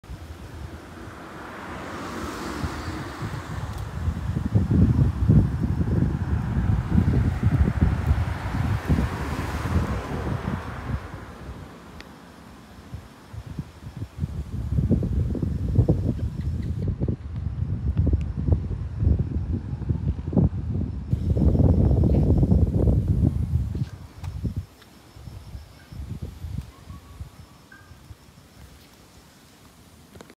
Zyxomma petiolatum - volans edit
Zyxomma obtusum Albarda, 1881 ?  volans edit

多分 オオメトンボ [おほめとんぼ]、日暮れの攝食飛翔